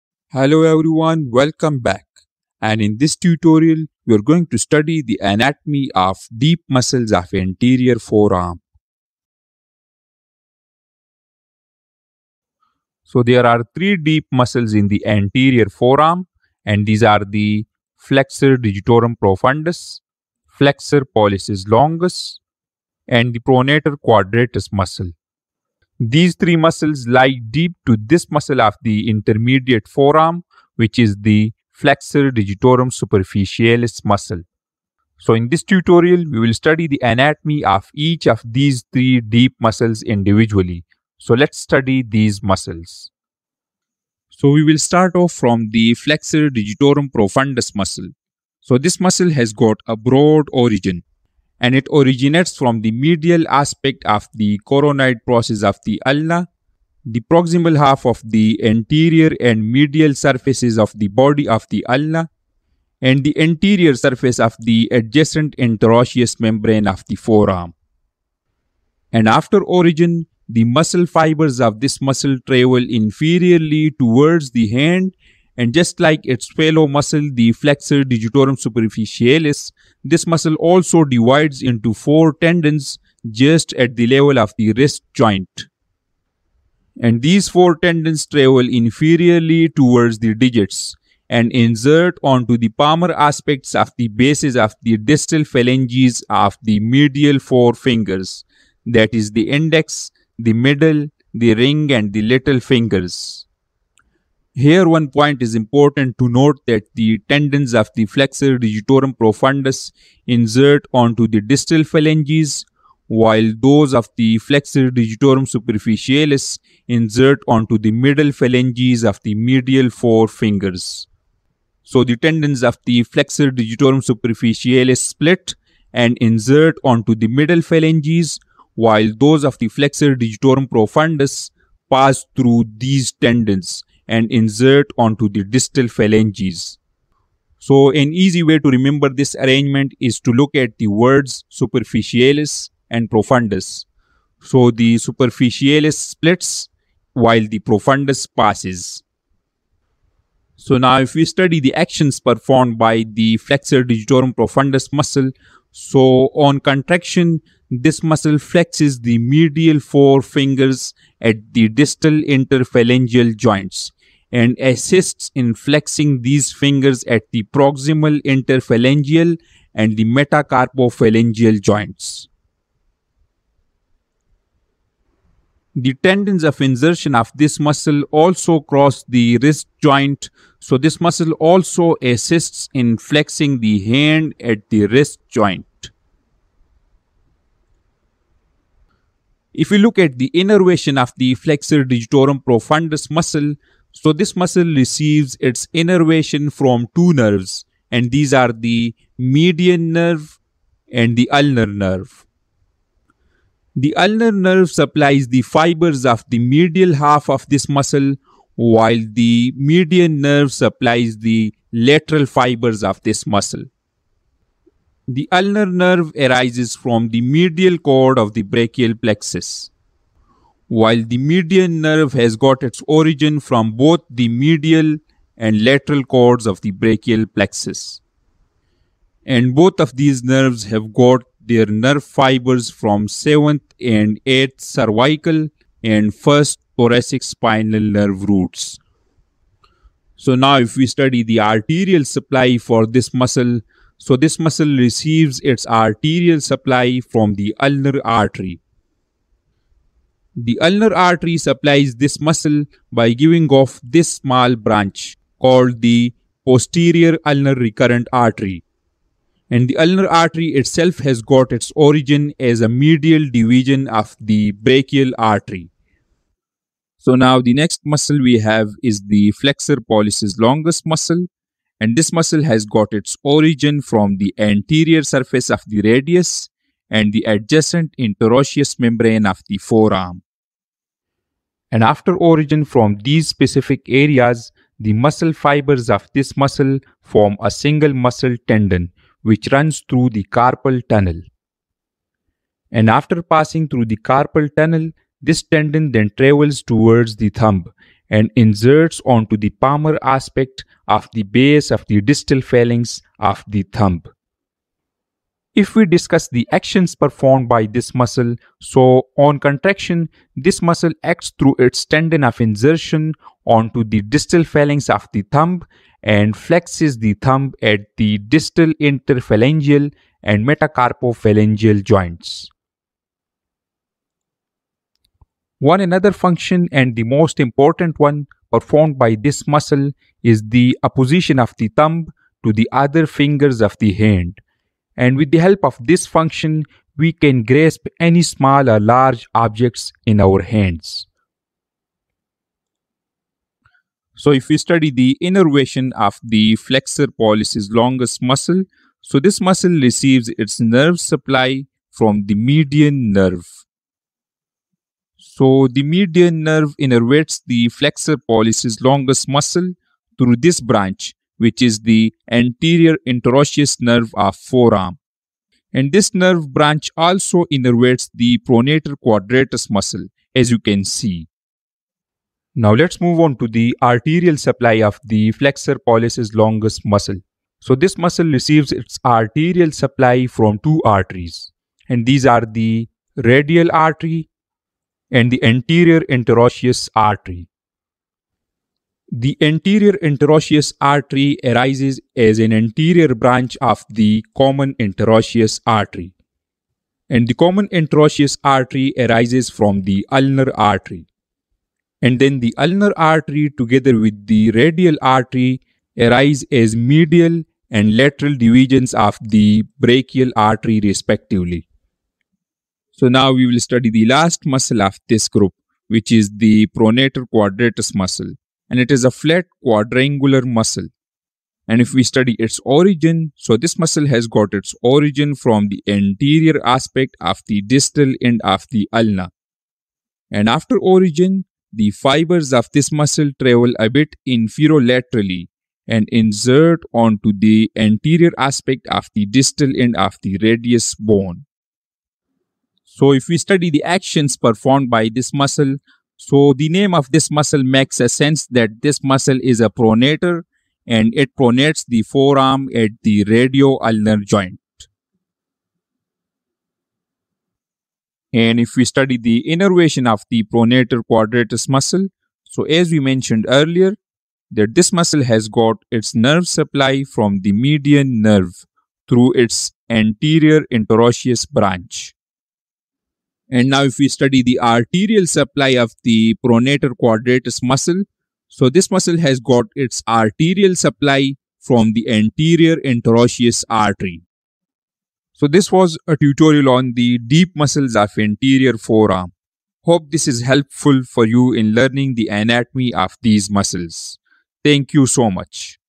Deep Muscles of Anterior Compartment of Forearm | Anatomy Tutorial | Doctor Speaks
Hi everyone!
Welcome Back!

This tutorial is about a brief overview of the Anatomy of Deep Muscles of Anterior Forearm. So there are three muscles comprising the deep part of anterior forearm and these are the Flexor Digitorum Profundus, Flexor Pollicis Longus and the Pronator Quadratus muscle. These muscles lie deep in the forearm and that's why these muscles can't be readily palpated. The intermediate muscle of anterior forearm, the Flexor Digitorum Superficialis, overlies these muscles.

So this tutorial elaborates the important anatomical features of these muscles.

Chapters
0:00 General Introduction and Location
0:43 1) Flexor Digitorum Profundus
4:26 2) Flexor Pollicis Longus
6:52 3) Pronator Quadratus

For similar muscle anatomy tutorials, follow the given links.